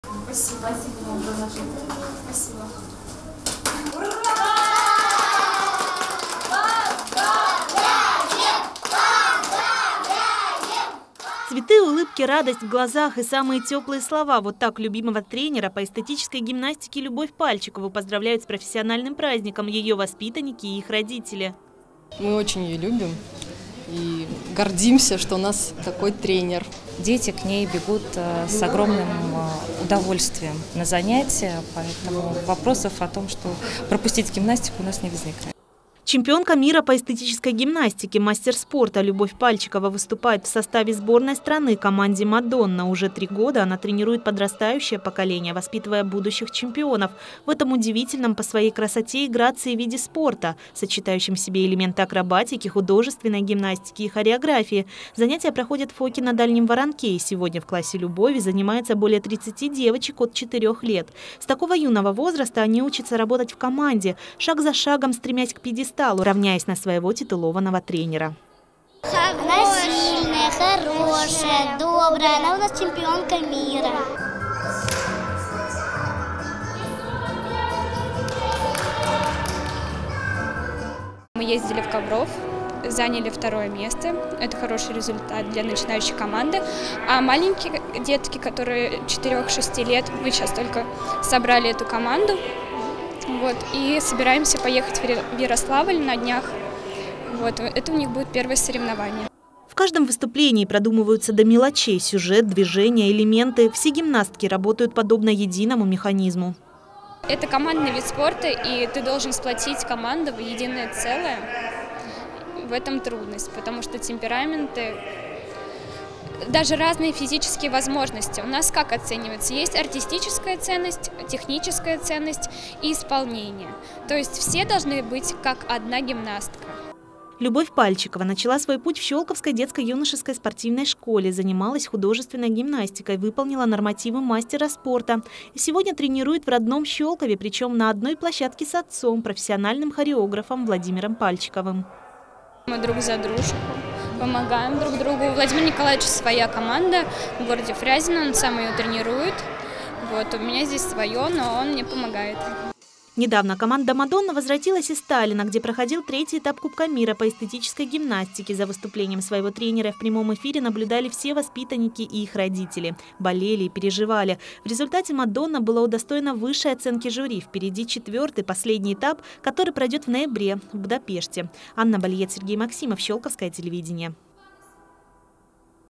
День тренера
30 октября в России отмечается день тренера, поэтому наш следующий репортаж будет посвящён человеку именно этой профессии. Мы расскажем о тренере по эстетической гимнастике Щёлковского Физкультурно-оздоровительного комплекса, мастере спорта и чемпионке мира в данном виде спорта Любови Пальчиковой. С ней, её воспитанниками и их родителями пообщалась Анна Балиет.